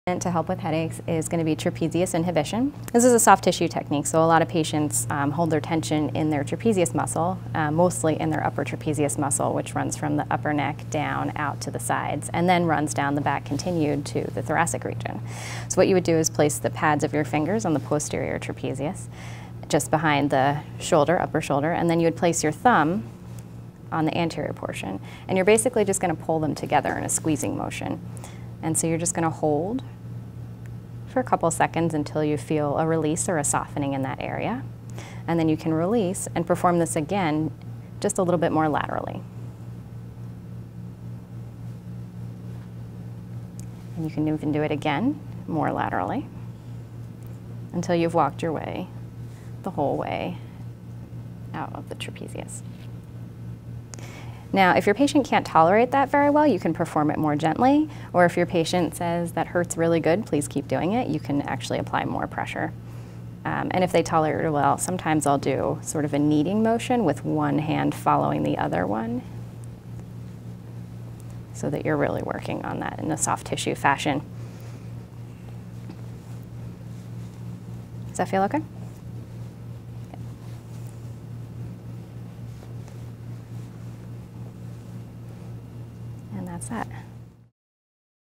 Trapezious Inhibition/Soft Tissue
2019 OMT Demonstration Videos - 1 of 4
Rowan University School of Osteopathic Medicine

Trapezious Inhibition/Soft Tissue - OMT Techniques

Catherine M Fusco D.O.
Family Medicine, NMI Osteopathic Manipulative Medicine
One Medical Center Drive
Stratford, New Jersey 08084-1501